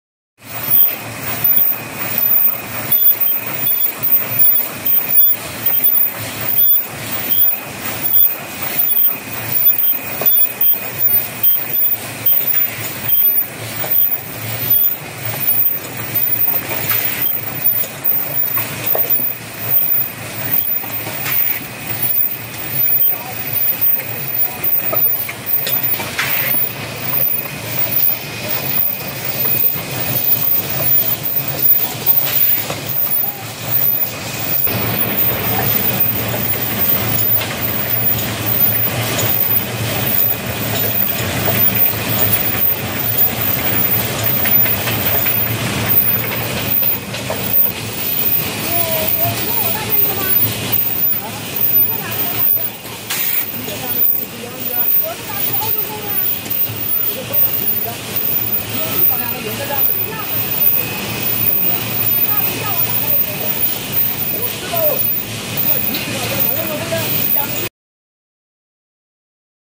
High speed automatic paper cup packing machine working with 4 forming machine
Our factory have packaging solutions for over 20 years